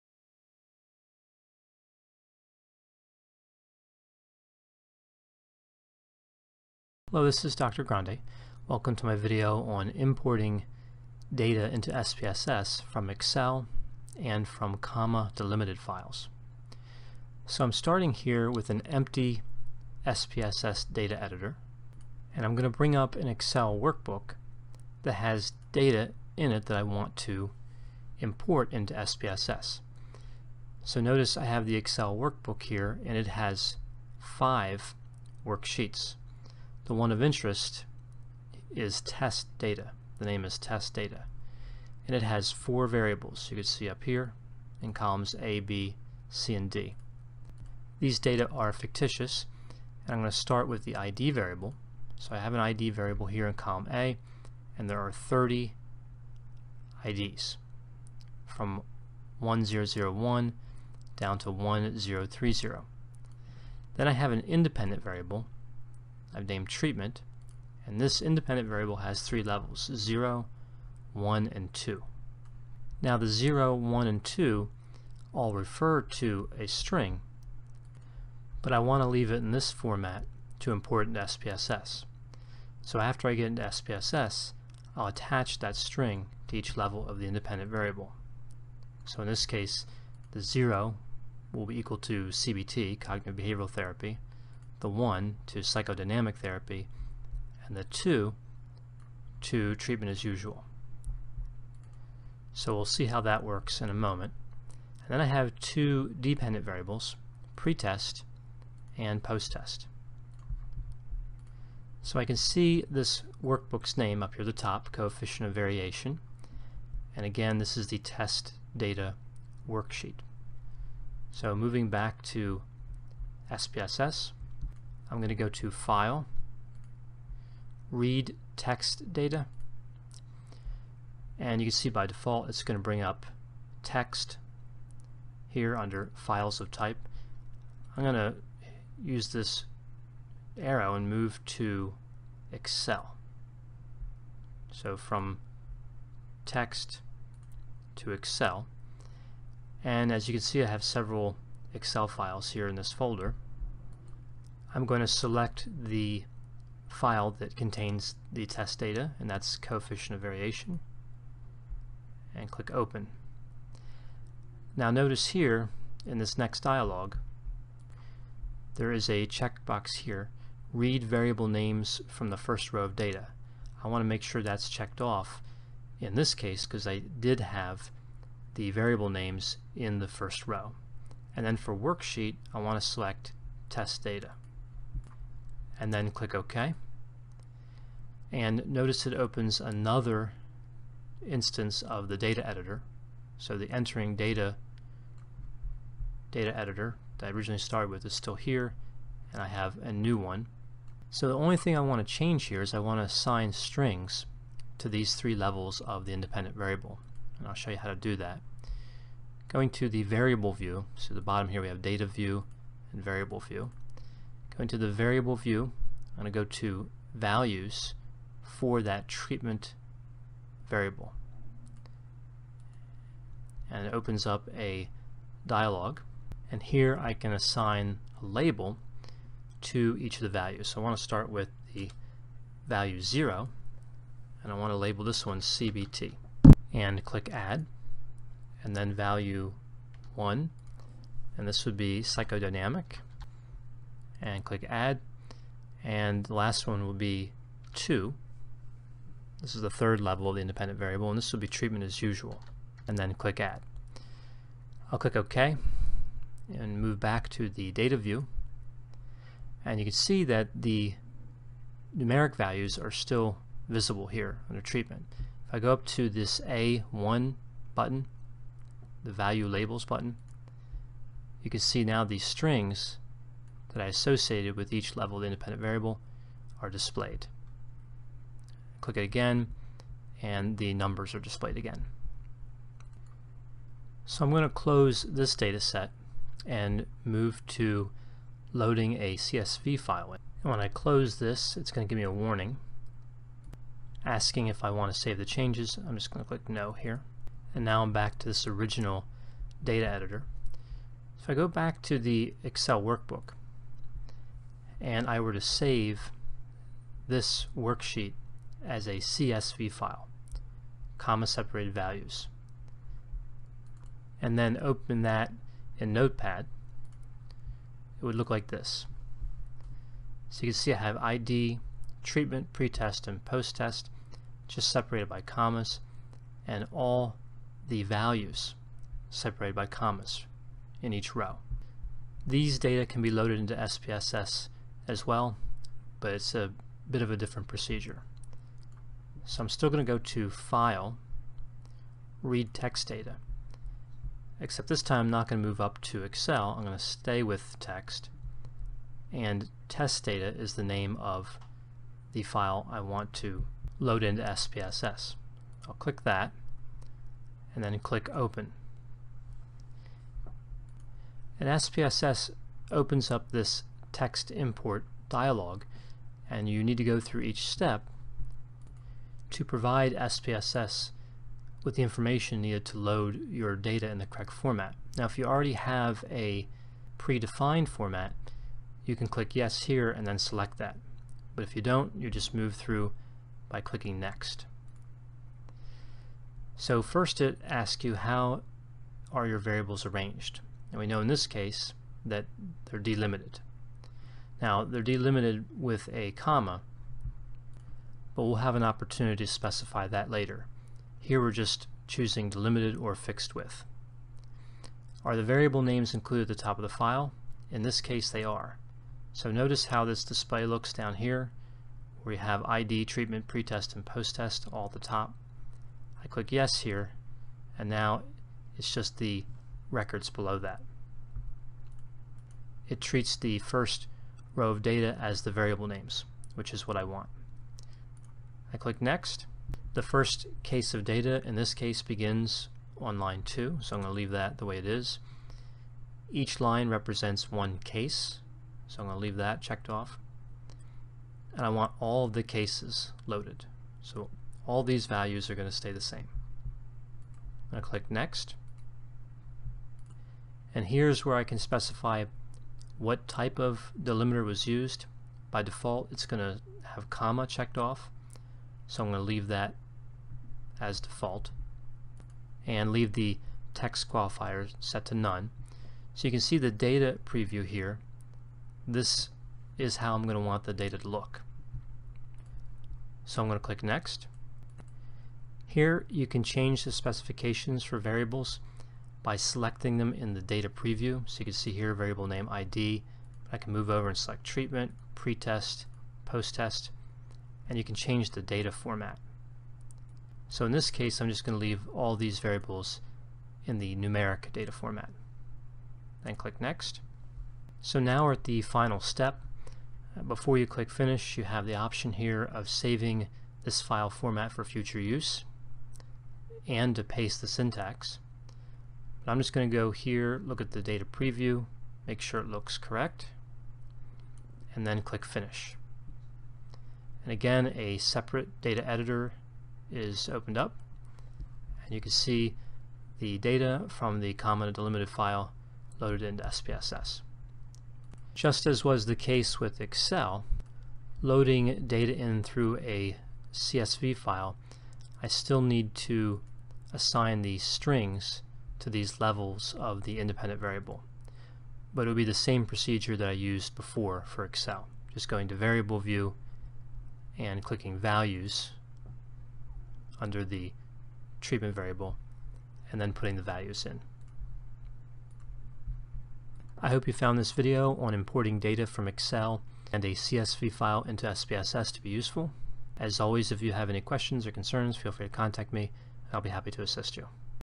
Importing Data from Excel and Comma Delimited Files (csv) into SPSS
This video demonstrates how to import data from Microsoft Excel and Comma Delimited Files (.csv) into SPSS. A procedure for assigning labels to the numeric values of the independent variable is reviewed.